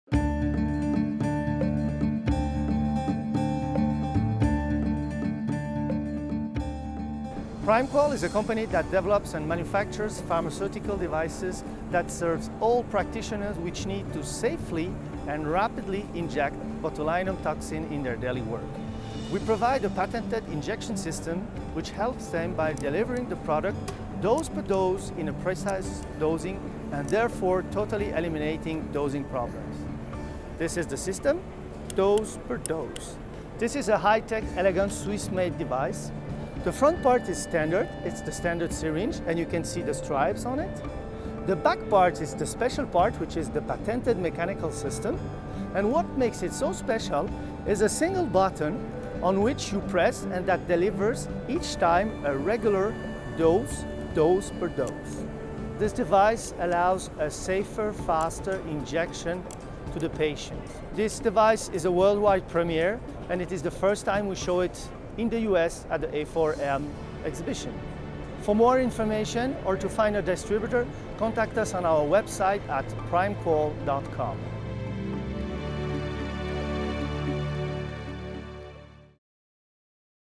Primequal A4M Conference Talent BT Premier
Primequal is a Swiss specialist of injection devices (PFS) for pharmaceutical drug delivery. Talent BT is the world's first automatic injection device developed to enable a safer, faster, mastered and repeated dose delivery of botulinum toxin. Talent BT turns out to be a true and recognized step up in patient comfort.